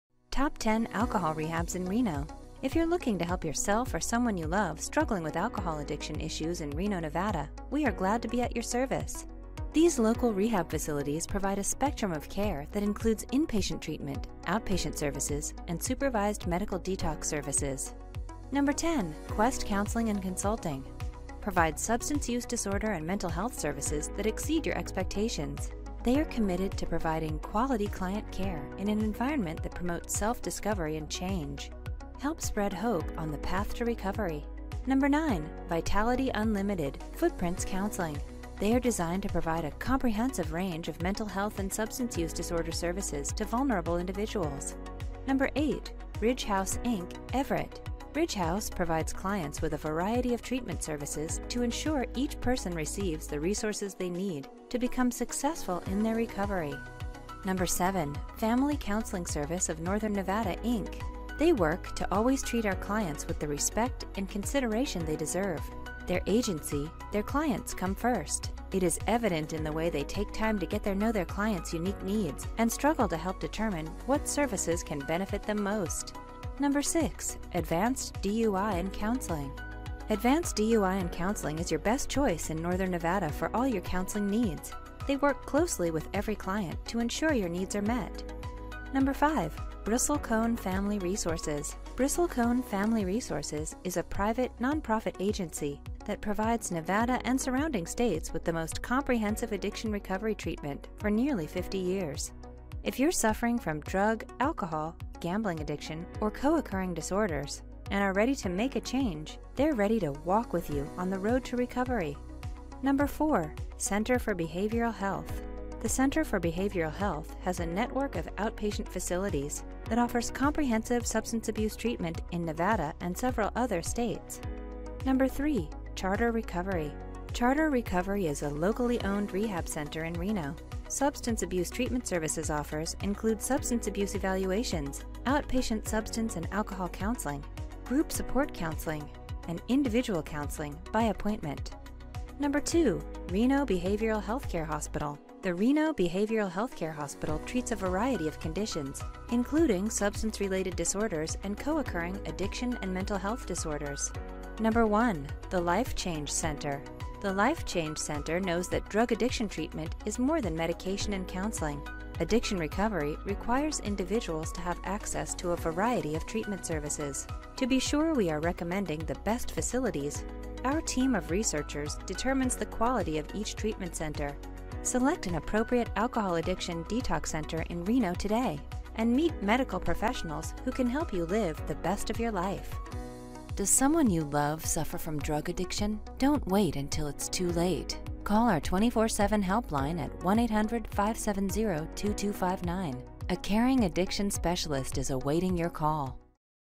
Alcohol Rehabs In Reno
These Alcohol Rehabs In Reno offer a wide selection of treatment services an addicted individual can undergo.

They have inpatient and outpatient programs and aftercare plans that contribute to a successful recovery. They also administer medically-supervised detox for drug-dependent patients.